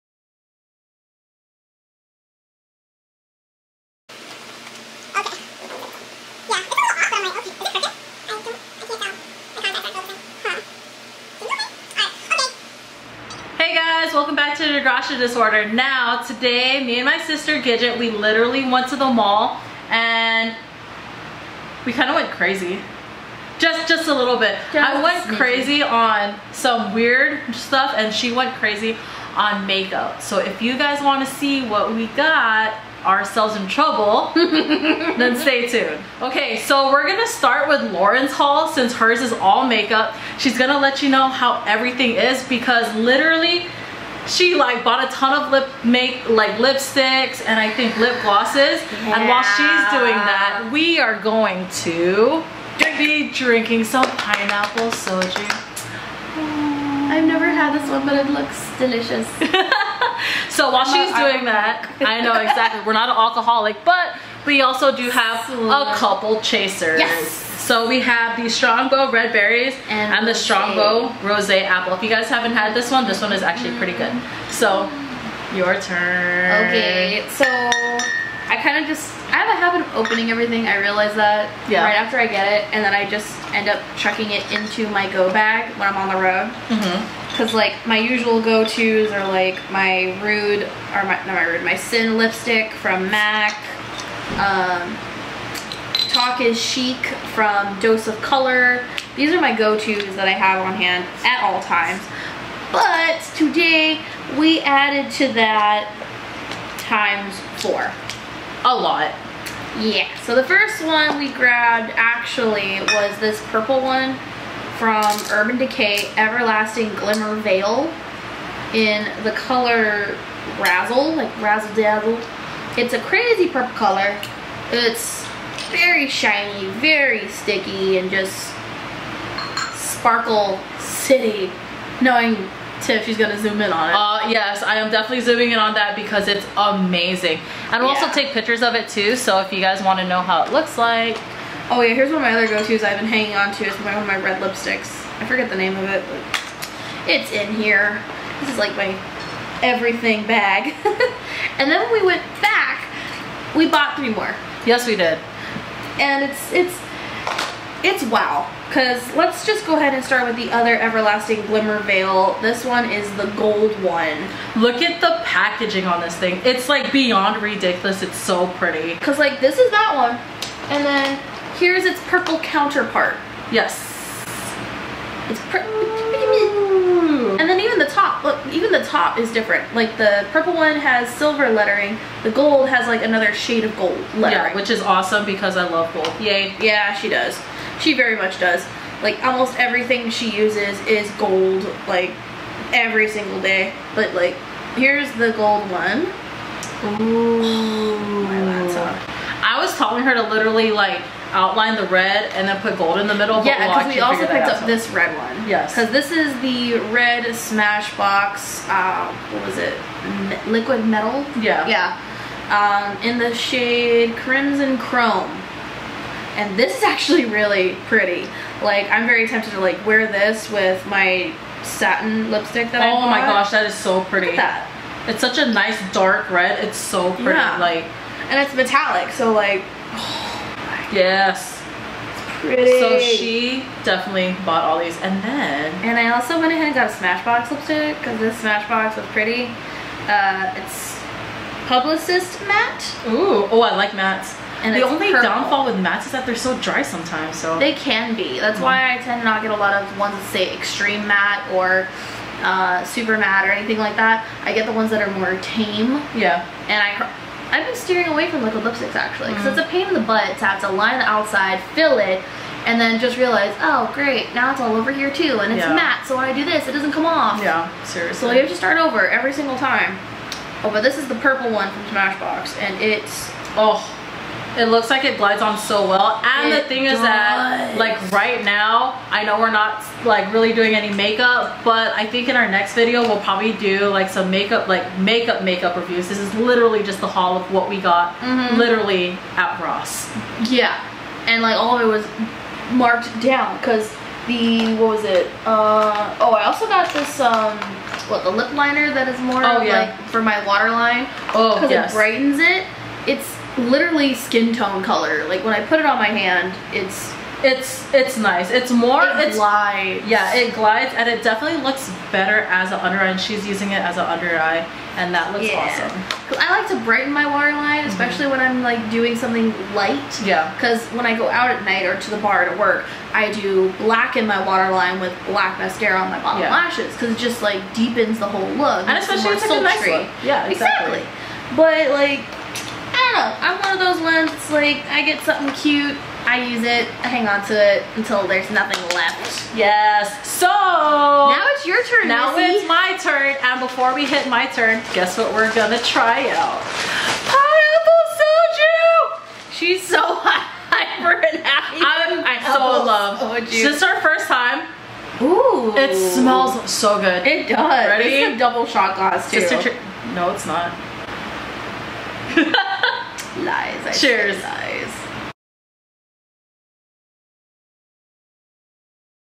degracia disorder Mall Haul (Ross)
LOREN'S HAULS

Hey guys! We always have a great day hanging out with each other. Gidget and I literally went into Ross (mistake number 1) and found the make up aisle! It great that we can literally go bargain hunting to support our make up addiction! Add some Soju fun and call it a wrap!

Love ya

Shout out to:
Sarah Jane IG @jane.lash.beauty FB: Jane Lash Beauty
EnEWear Eric Camacho IG @enewear

Loren aka Gidget
YouNow: gidgetbabe